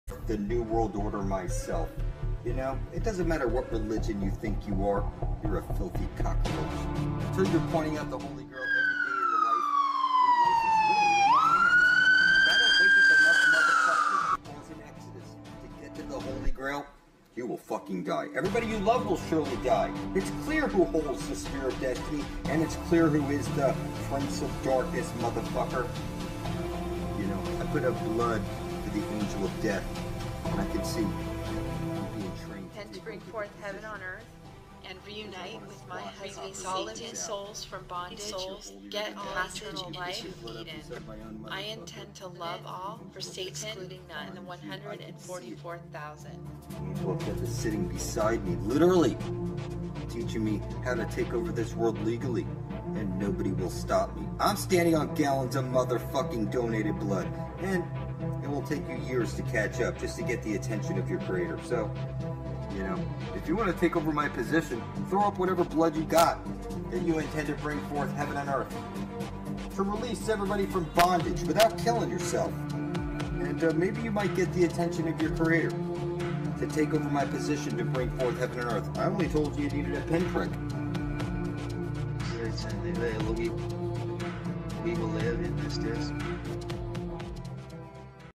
Mother Goddess - Blood Over Intent Heaven On Earth
Blood Over Intent Saves the Universe
II Corinthians 4:4 Satan Owns the Universe.
"The Universe is mine. All your leaders swear on a Bible- II Corinthians 4:4 phuck you, the Universe is Mine." -Satan, King of Hell, Mark Braun, The Devil in the Flesh
Satan opened the Book of Life and it has the names of 144,000 Living Beings

144,000 Blood Over Intent Family are Satan's Sealed Elect Bringing Forth Heaven on Earth for the Benefit of All to release all from bondage. Wake Up and get blood thick. Leviticus 17:11 The Power is in the Blood.